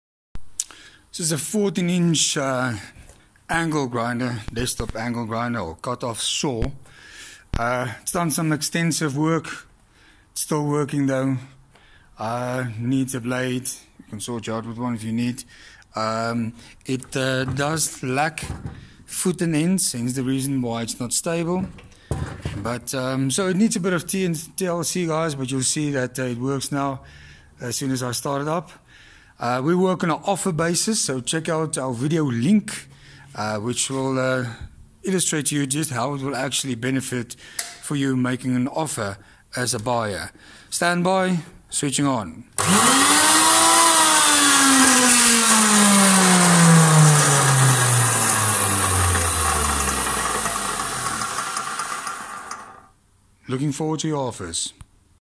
14-Inch Portable Cut-Off Saw - 2200W
Details
Condition: Used - Fair
Item may have some signs of cosmetic wear, but is fully operational and functions as indicated.